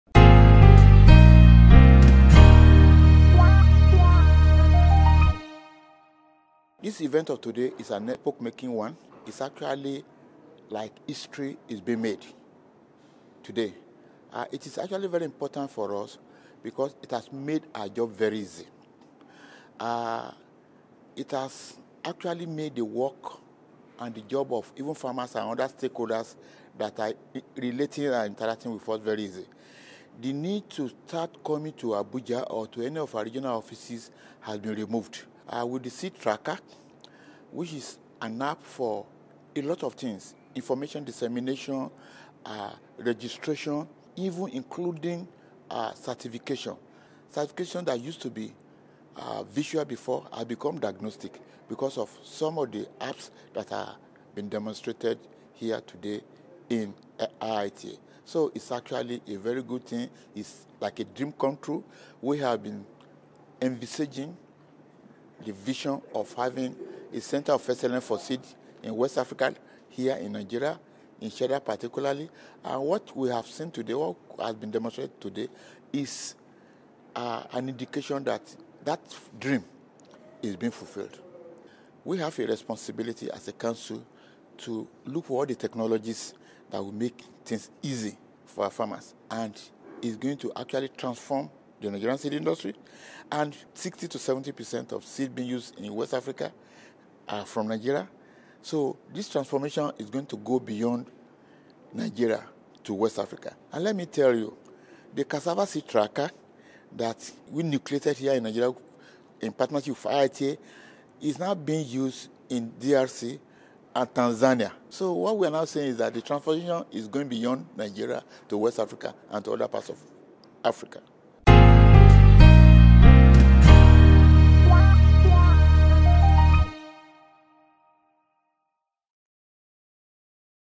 Interview with Dr Philips Olusegun-Ojo | IITA’s Smart Tools for Smart Farming.
IITA’s Smart Tools for Smart Farming
Promo Day for Digital Connections
1st November 2019